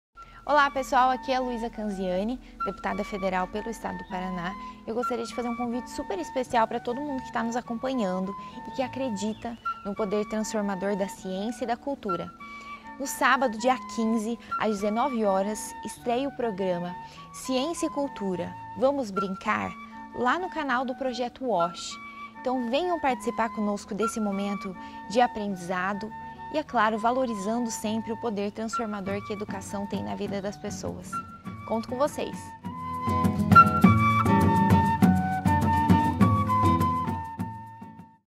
Deputada Luisa Canziani convida para o Ciência e Cultura, vamos brincar?
Vídeo de divulgação e convite para o lançamento  do Projeto Ciência e Cultura, vamos brincar?, apresentado  pela deputada Luísa Canziani, deputada federal pelo PR, apoiadora do Programa WASH, destinado ao público do PR.

Sobre o Programa

O programa "Ciência e Cultura, vamos brincar?" une diversos especialistas e parceiros para abordarem conteúdos relacionados à Ciência, à Cultura, às Artes e à Ludicidade, proporcionando o aprendizado com diversão.

Acompanhe o WASH

Acompanhe o movimento Nós Somos a Ciência

Acompanhe a Cia Bola de Meia